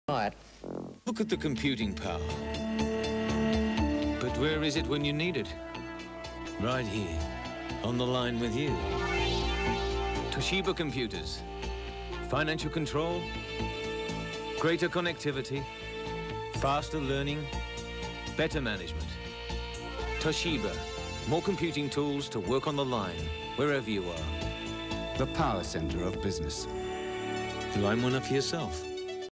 Toshiba Laptop commercial - Australia 1990
Toshiba Laptop advertisment from the early 90s. These computers were state of the art acording to Toshiba.